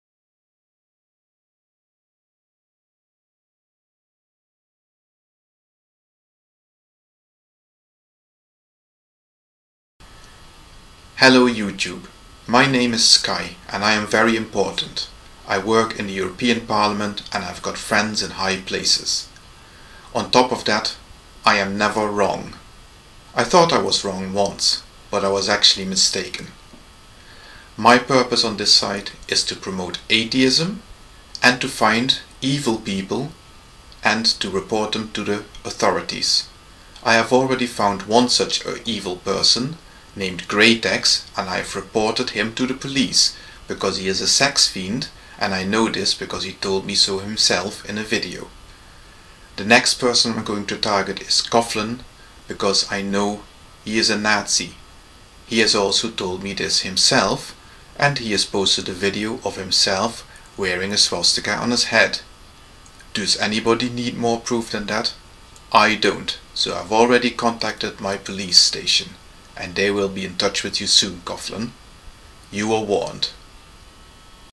90 second pwnage ... limitless arrogance
I refer to gratex' video simply because it's the only place in which Sky's immortal footage still survives. Here is a man who takes himself deadly seriously. And everyone else too. Apparently.

A worthy cause for pwnage, indeed.